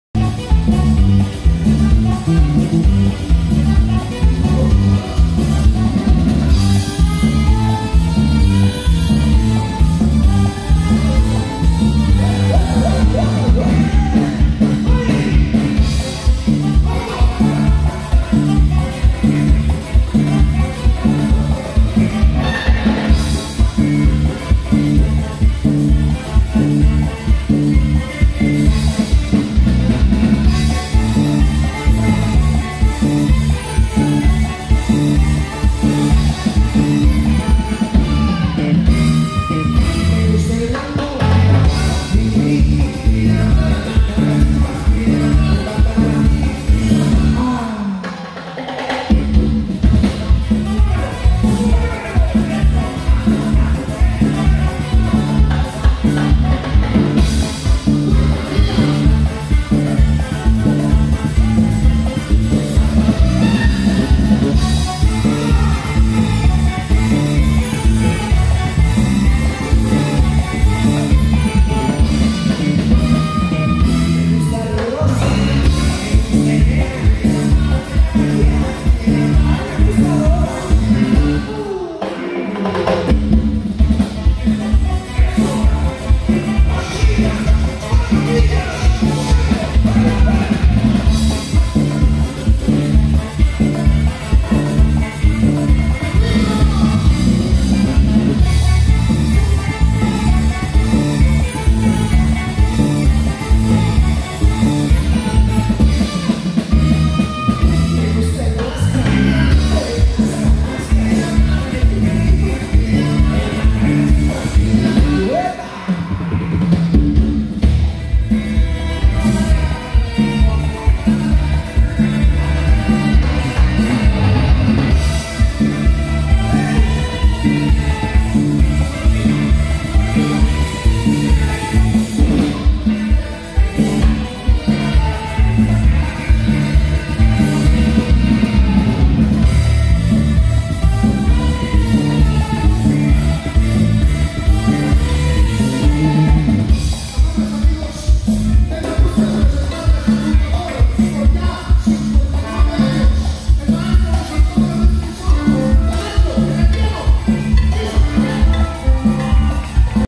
Los Socios Del Ritmo.
Nacidos en la ciudad de Campeche, Campeche, el 12 de noviembre de 1962, la Organización Musical LOS SOCIOS DEL RITMO se han mantenido en el gusto del respetable por más de cinco décadas ininterrumpidas.